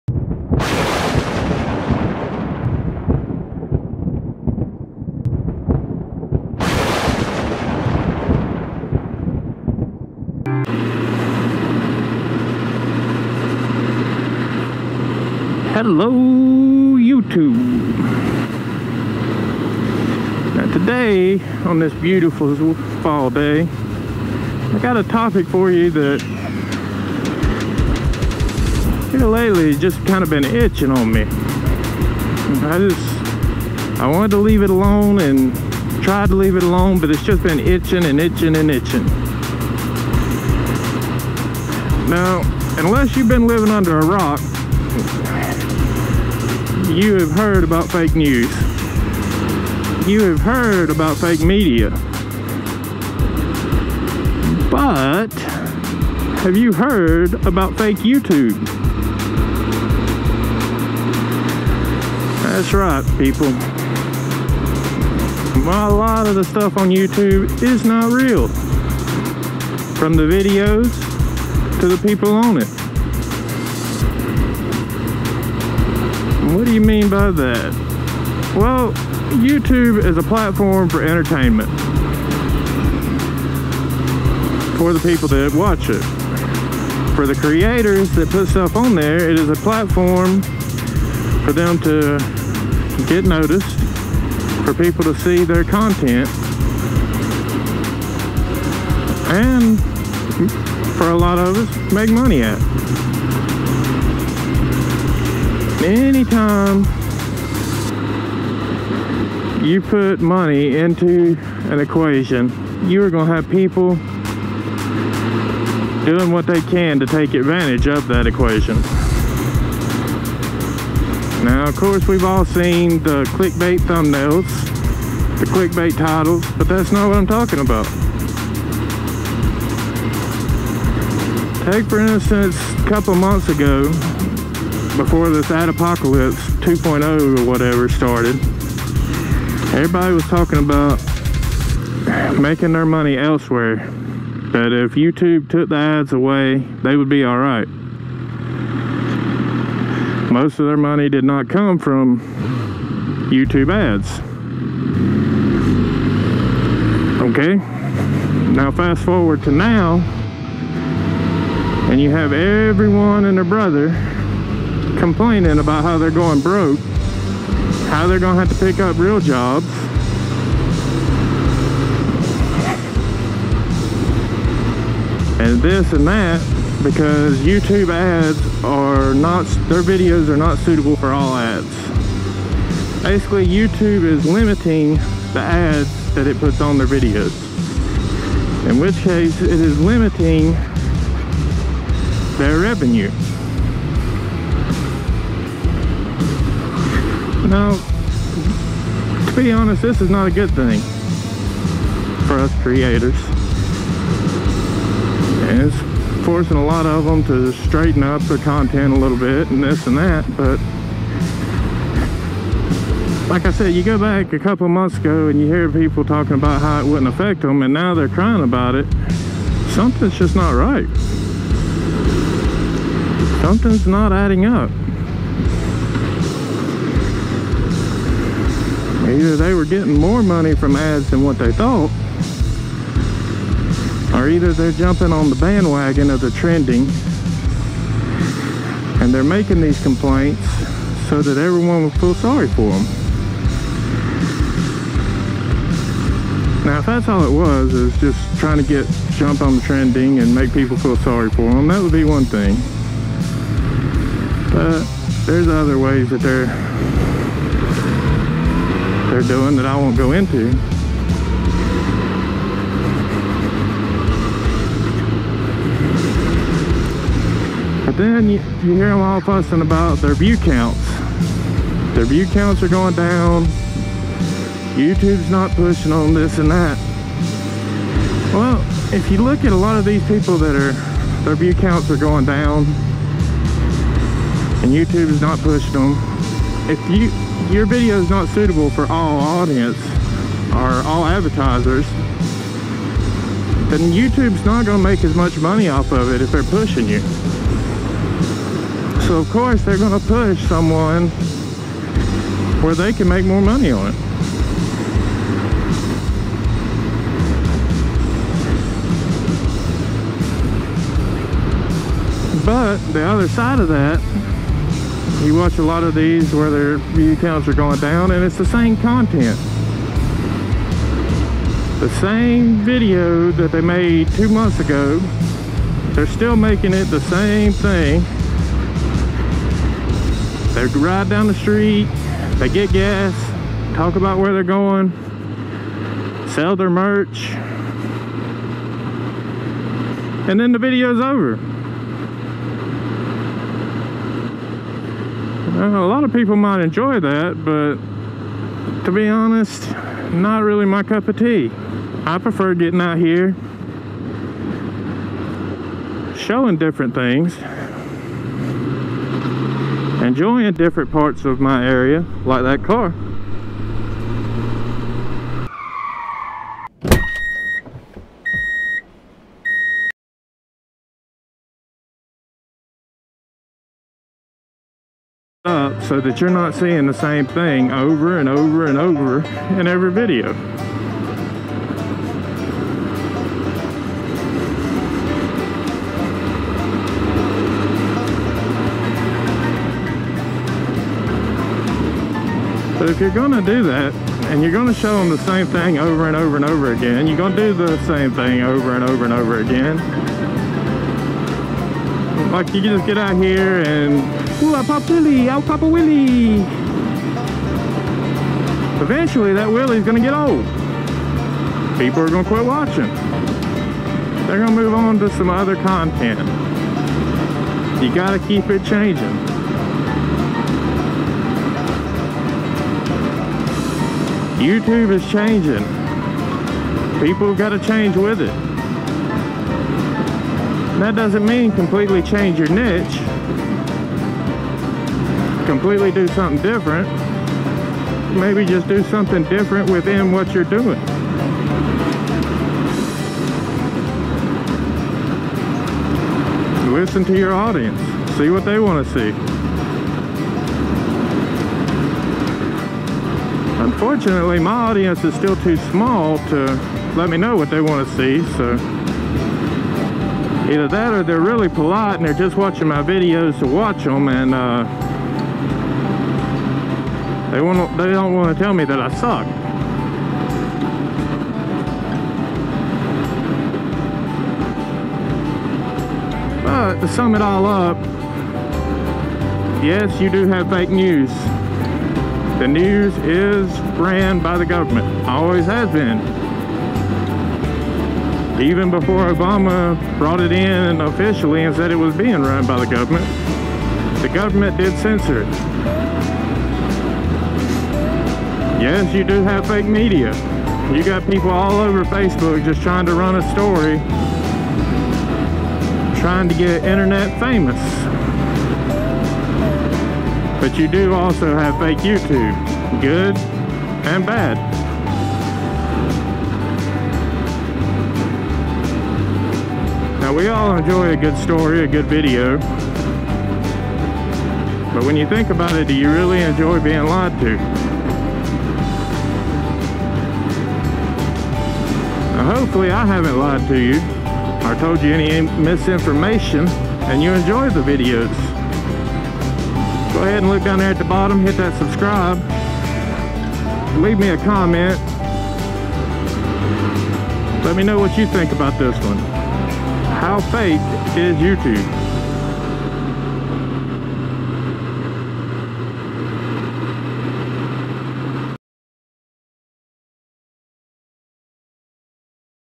Fake news, Fake media, Fake YouTube. Is anything real anymore?
Need a hoodie, a shirt, or perhaps a sticker grab yours @

We all know almost all of the news is censored and controlled. We've all seen the fake news reports, or biased news. Fake media such as photo shopped Instagram to bogus Face Book stories looking for 15 seconds of fame, but what about fake YouTube? Some videos are fake for entertainment, some are fake just to lead you to a false conclusion, and some are just down right false.